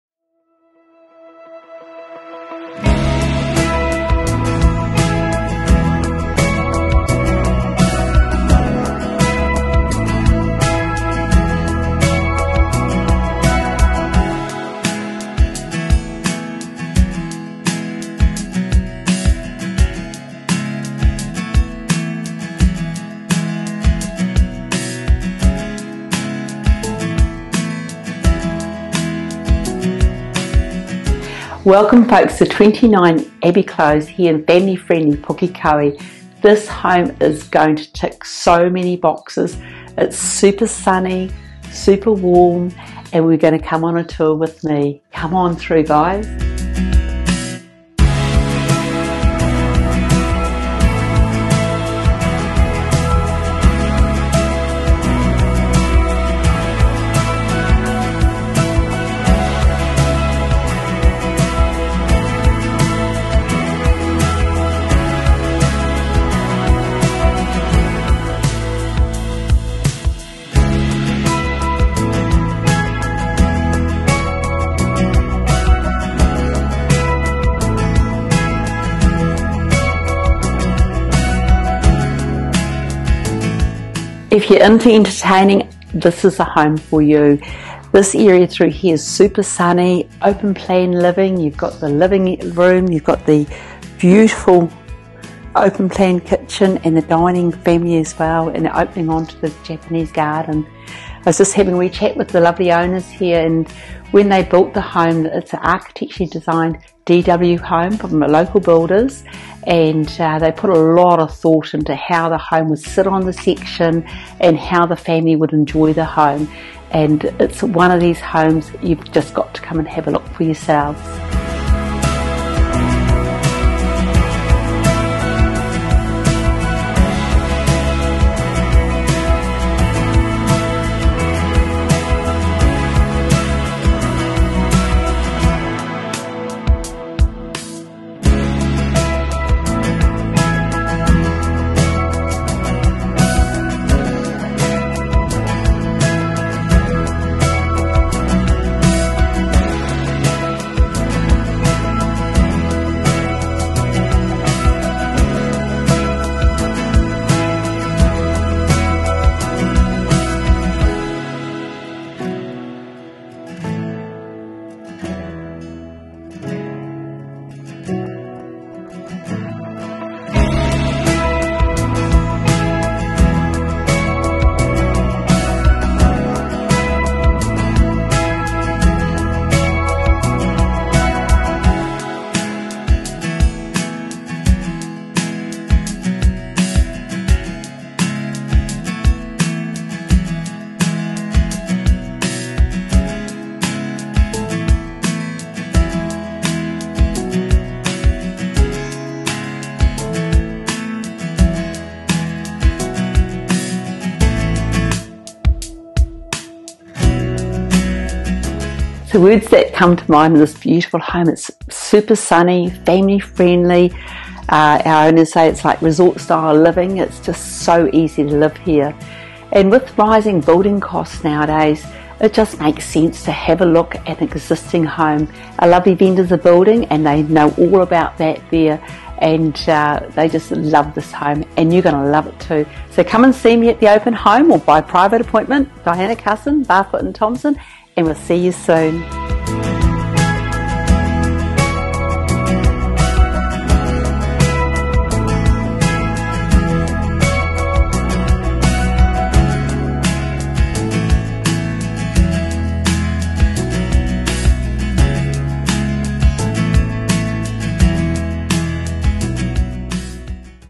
SOLD - 29 Abby Close, Pukekohe - Diana Cussen
ELEVATED LUXURY LIVING, PRESTIGIOUS LOCATION

Triple car garaging.

Buyers price range: Mid $1 million plus

Surrounded by quality homes and views, this elevated, well-appointed, 288 square metre home is a real beauty. It is located in ...

Property Marketed by: Diana Cussen, Pukekohe